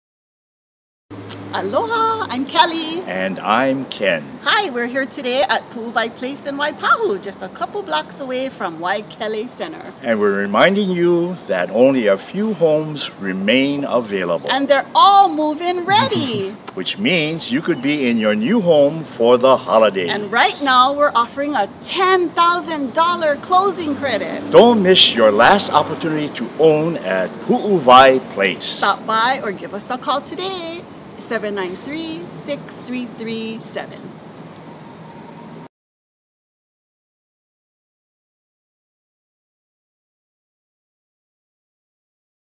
Puuwai Place in Waipahu - Attached Condominiums for Sale
Pu'uwai Place is a new Hawaii real estate neighborhood featuring 3 and 4 bedroom attached condominium homes conveniently located blocks away from Waikele Center and offering some of Oahu's best shopping, dining and recreational activities. for details.